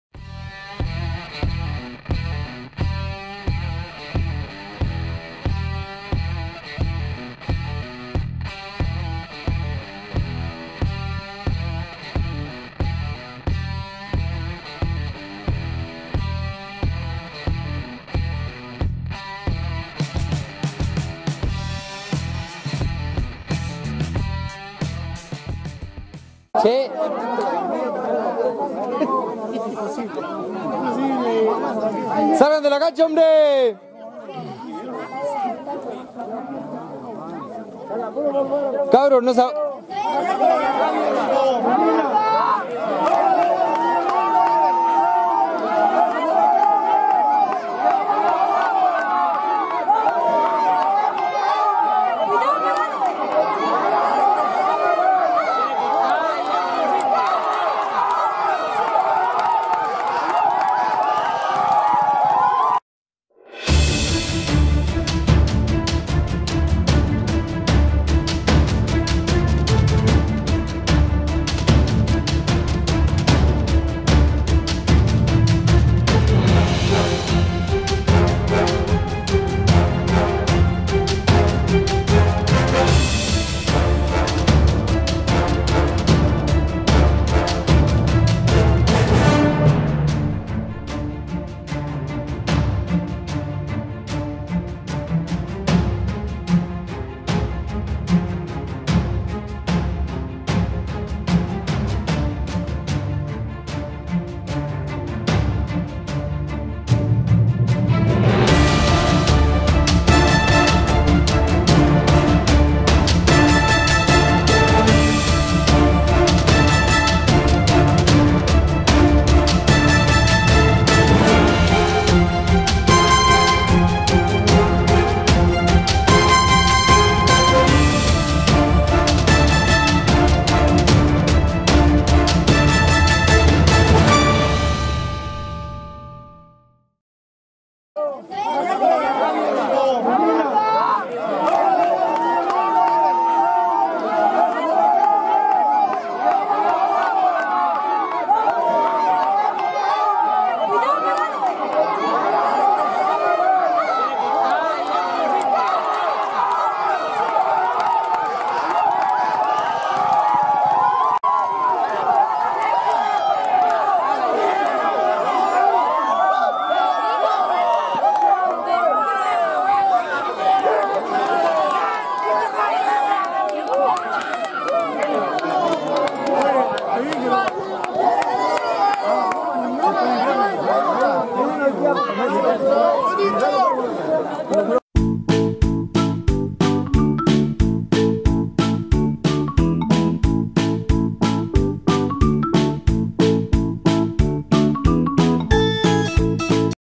Fatmagul vs Don Domingo - 400 Metros
Carreras a la Chilena - Complejo Hípico Chorombo